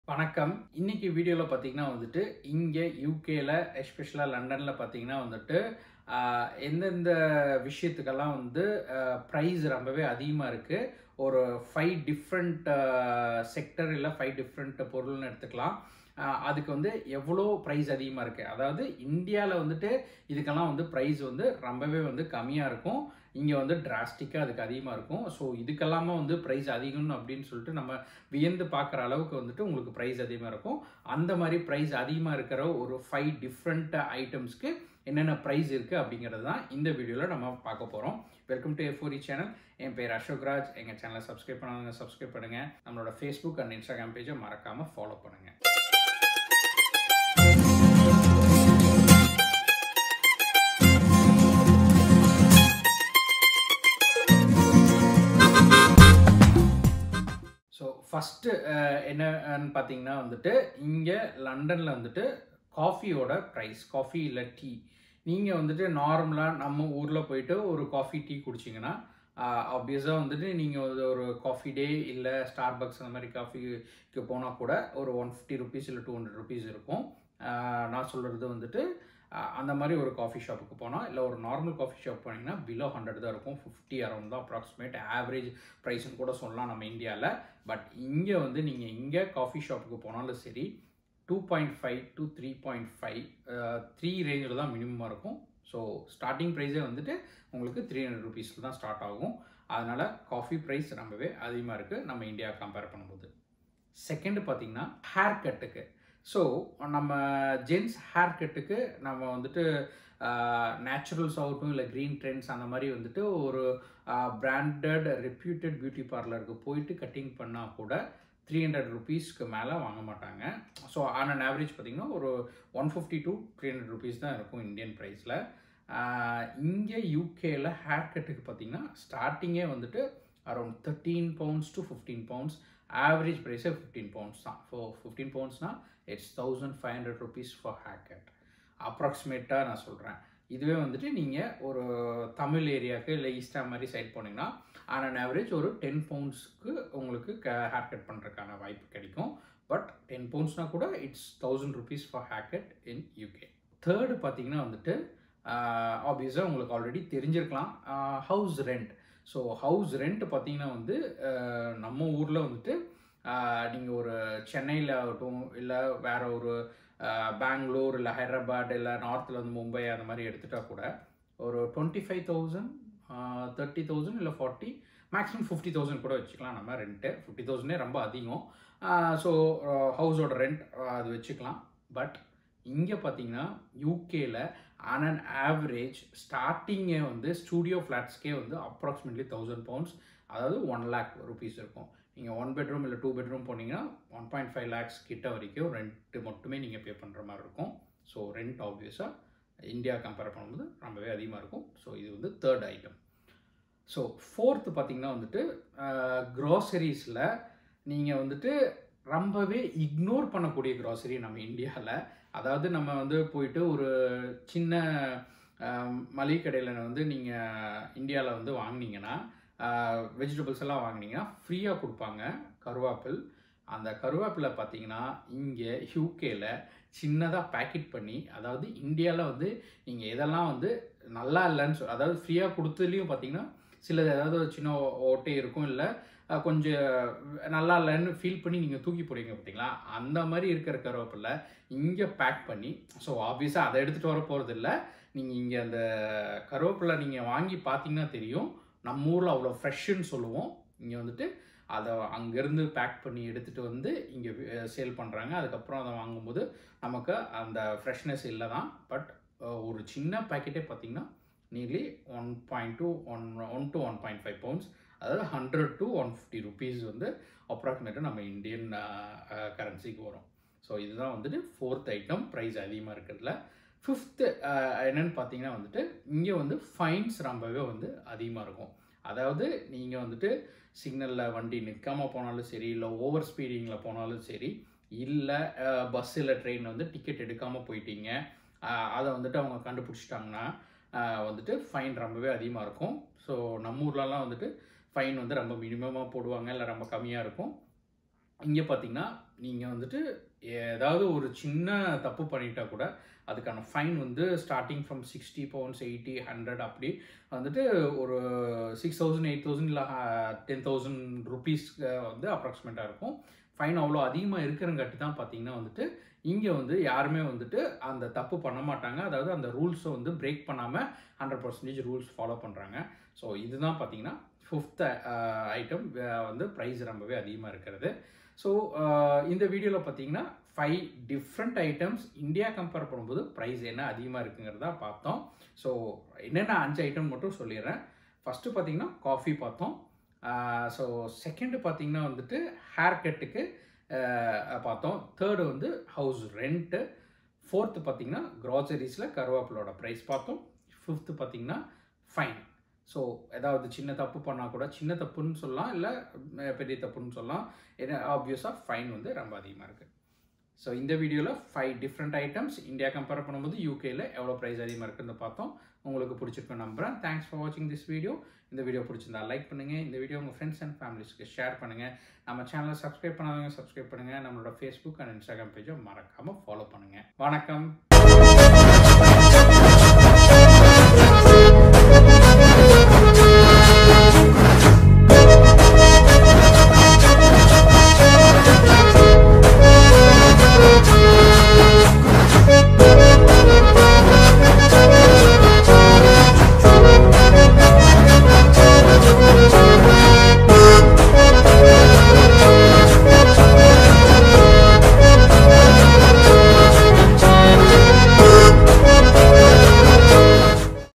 5 Interesting High cost items in London | London Tamil | A4e Unique Platform
For personal Guidance (Minimum Paid service)